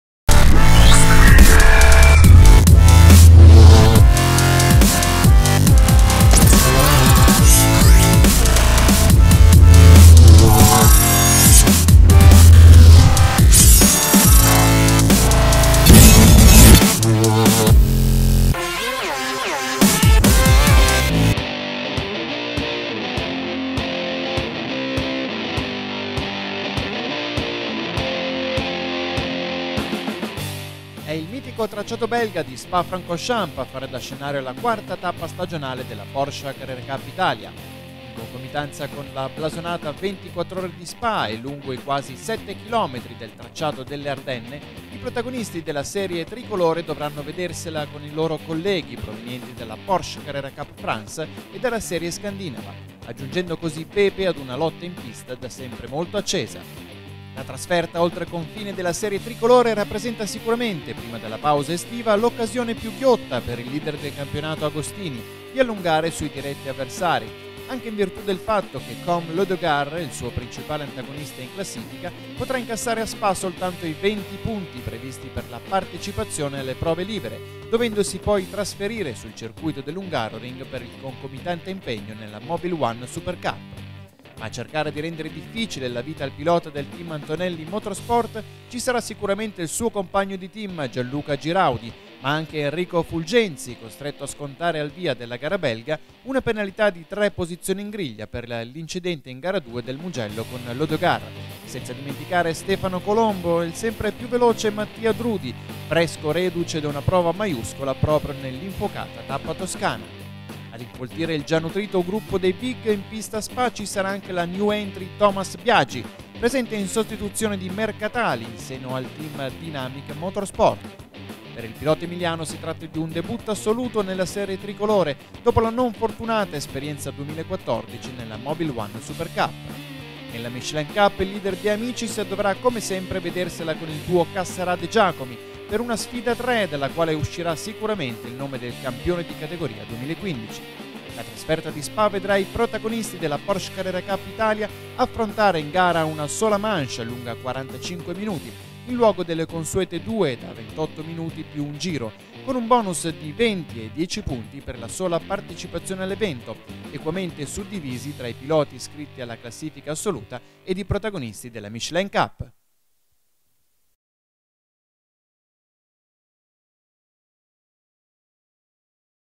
TERZO TEMPO - PCCI 2015 - INTRO TO ROUND 7 SPA
La Porsche Carrera Cup Italia si trasferisce all'estero per il 7° round stagionale sullo splendido tracciato di Spa Francorchamps in Belgio. Una panoramica sull'appuntamento che vede coinvolti 37 piloti tra campionato italiano, francese e scandinavo.